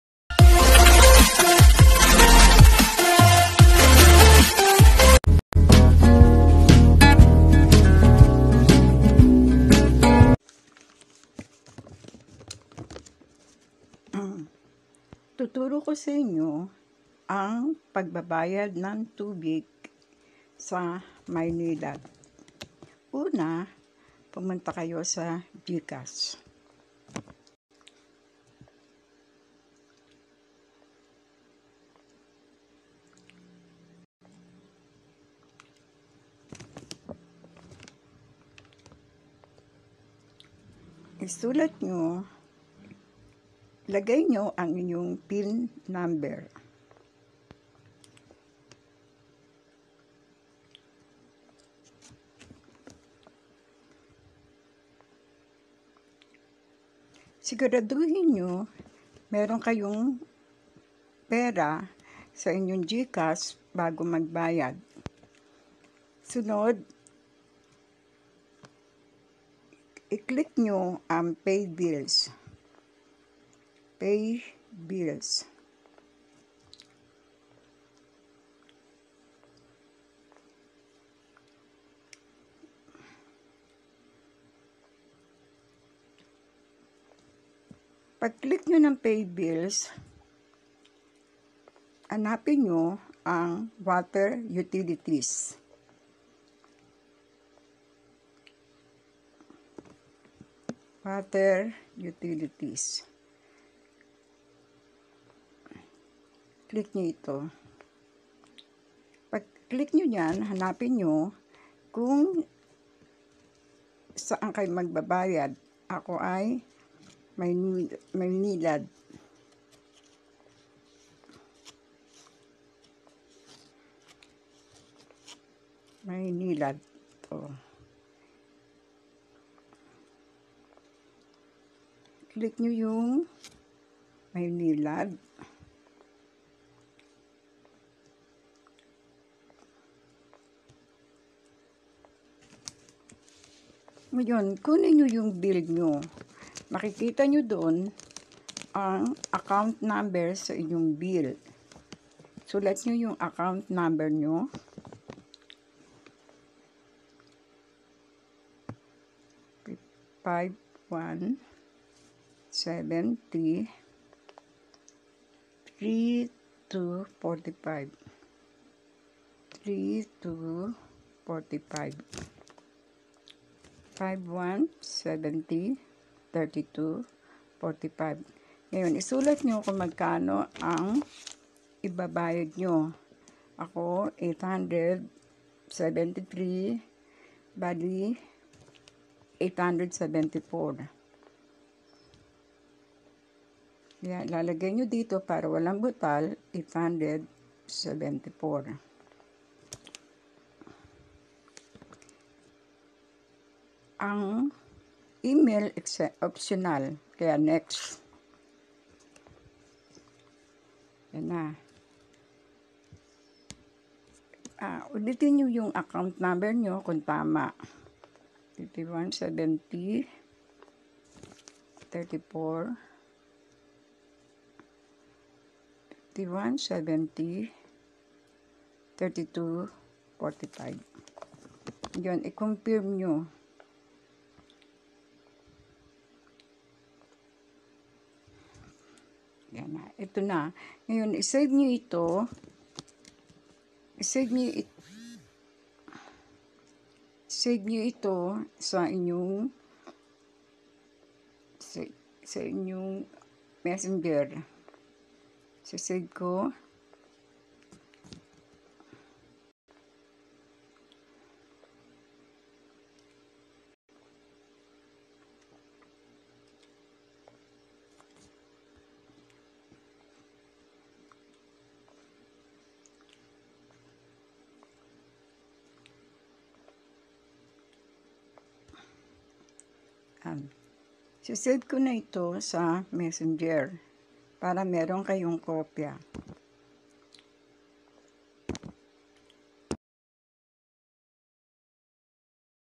Paano ang pagbabayad ng tubig sa Maynilad sa pamamagitan ng G-Cash
Pagsasabi ng hakbang sa pagbabayad ng tubig sa Maynilad sa pamamagitan ng G-cash